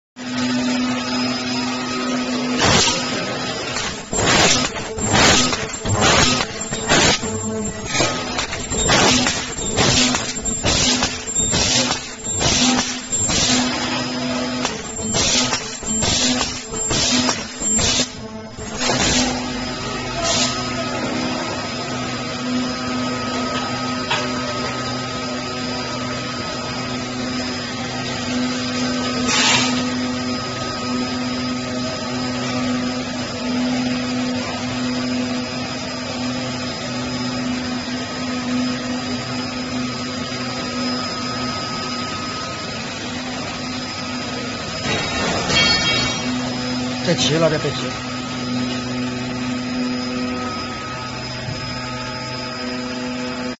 Ultrasonic seat belts cutting & hole punching machine, webbing belt cutting & hole punching machine
This high speed ultrasonic woven tape cutting machine is suitable for cutting and punching all kinds of washing water mark, sticker trademark, woven label, printed label, ribbon, non-woven fabrics, PVC trademark, etc. Featuring high speed and accurate feeding motor, the speed can reach 150 times / min, and the accuracy can reach 0. 3mm at a cutting length of 2.5m.